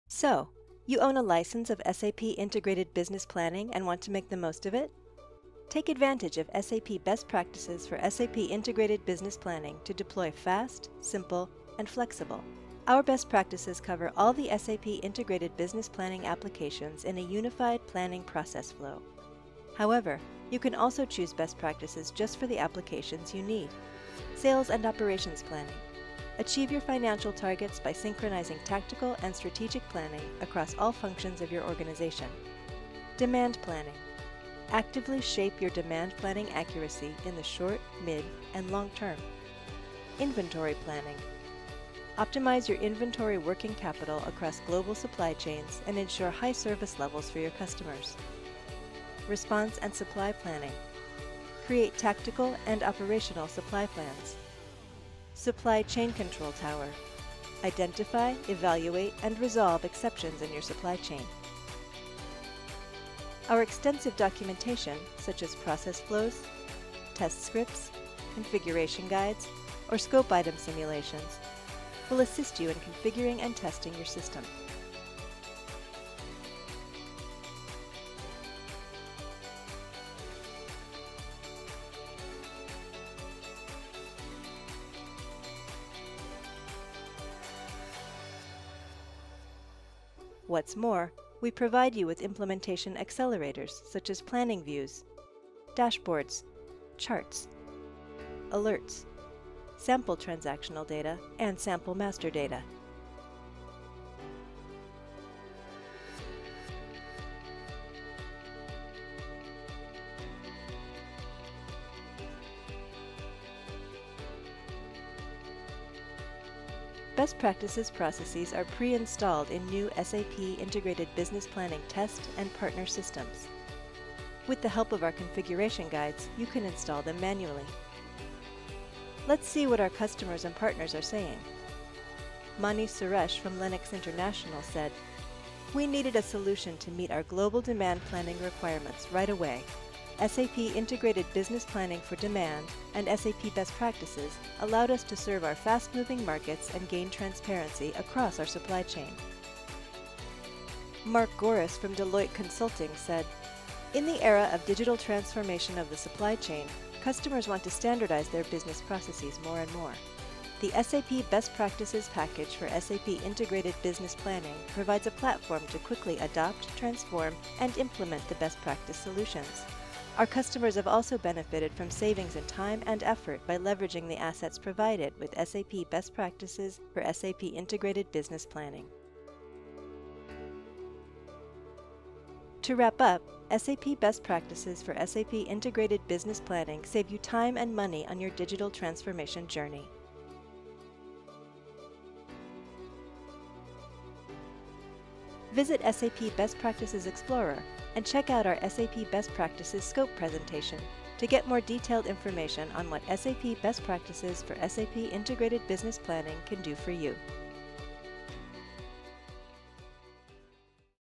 Take Off with SAP Best Practices for SAP Integrated Business Planning
So, you own a license for SAP Integrated Business Planning and want to make the most of it? Take advantage of SAP Best Practices for SAP Integrated Business Planning to deploy fast, simple and flexible. In this video, we’ll give you a short overview of what’s in it for you.
At its core, this is a comprehensive data model which enables you to execute an integrated, end-to-end business process. SAP Best Practices comprises planning view templates for interactive simulations and what-if analysis, as well as predefined dashboards for embedded analytics. Add to all of this SAP Jam integration for context-aware social collaboration, alert definitions for exception-based planning, and last but not least, sample data to showcase the end-to-end process.

Want to learn more? Go to SAP Best Practices Explorer